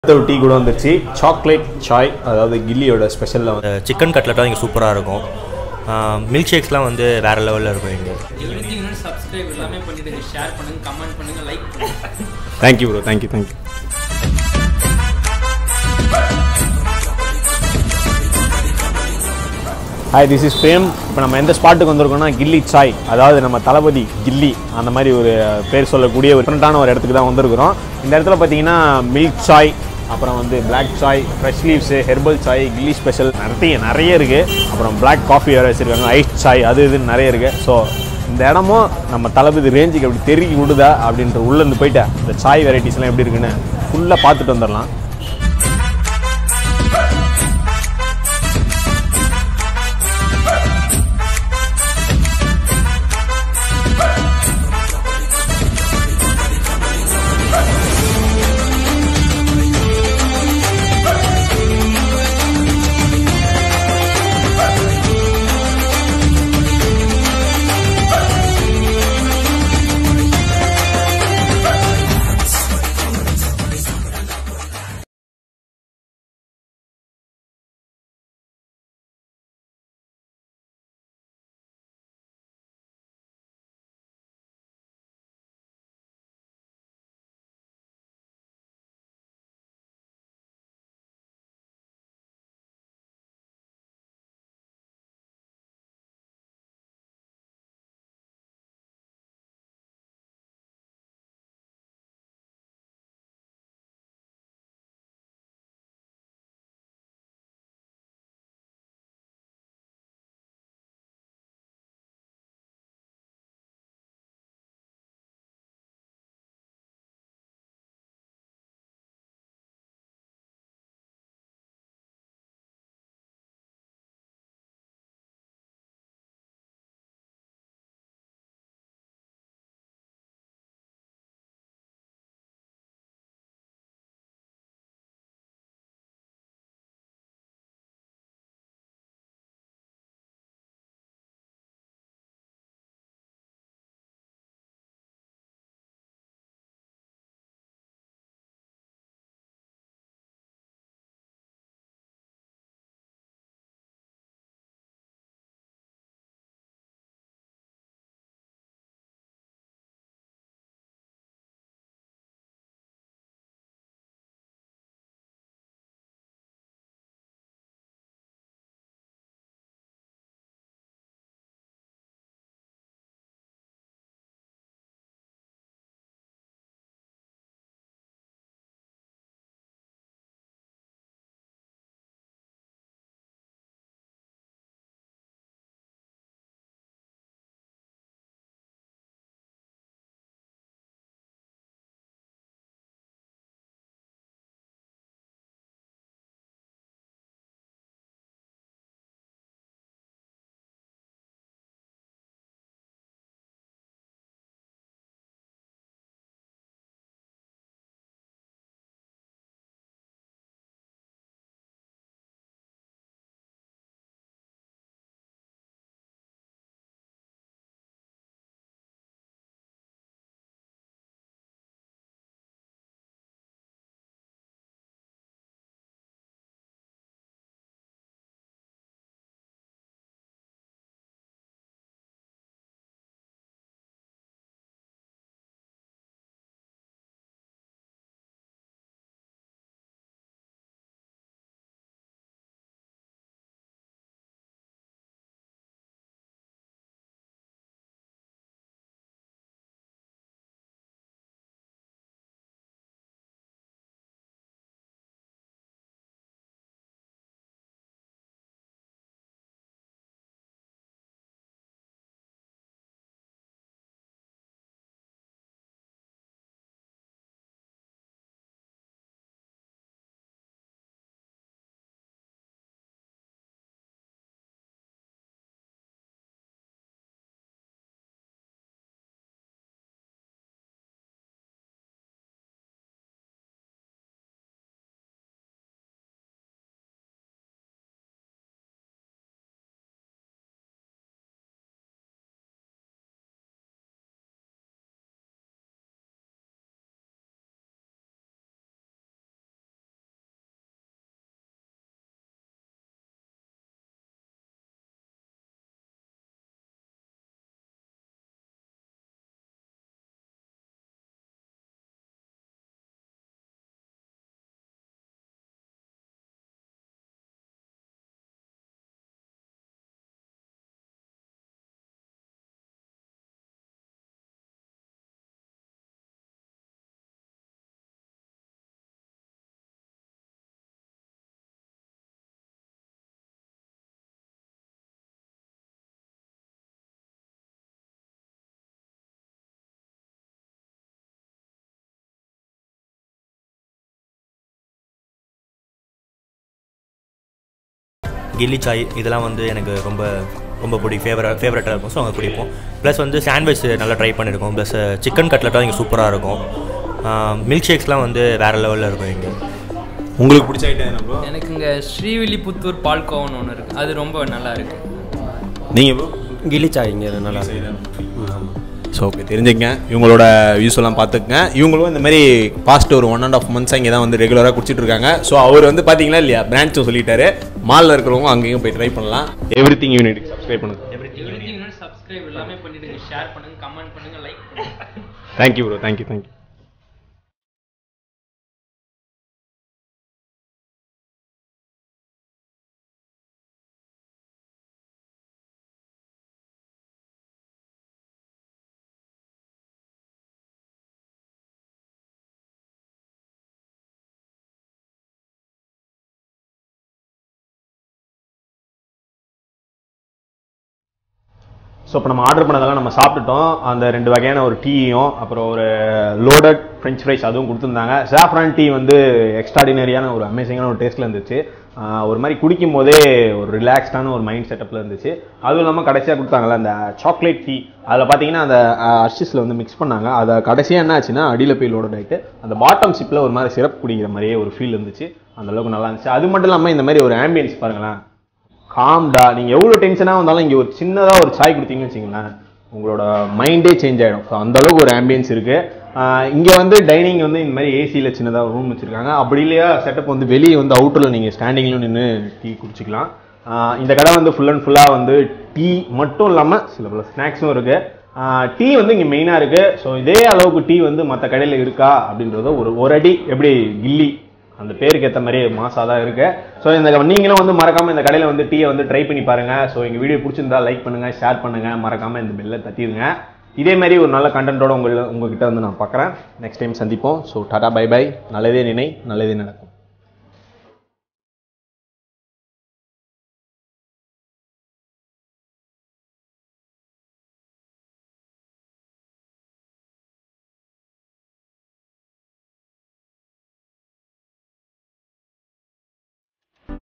GILLI CHAI - கில்லி சாய் in Pondicherry | Tea shop
Location : No 131, Mission St, Heritage Town, Puducherry, 605001

Gilli chai is a well reputed Tea franchaise in the world.  It originated in Dubai and having a huge fan base there.
In chennai there are some 5-6 outlets but in Pondicherry first time in India it’s an master franchise.

TIMELINE:
0:00​ - In This Video
0:20 - Intro
0:46 - Inside the restaurant
1:44 - Feeling the Tea
3:10 - Making of chocolate tea
3:24 - Enjoying French fries and chocolate tea
5:20 - Response from General Audience
6:52 - Closure

Cast and Crew  :
Insta handles: @RajFX , @Spartanprem, @GeraldMack, @Rajesh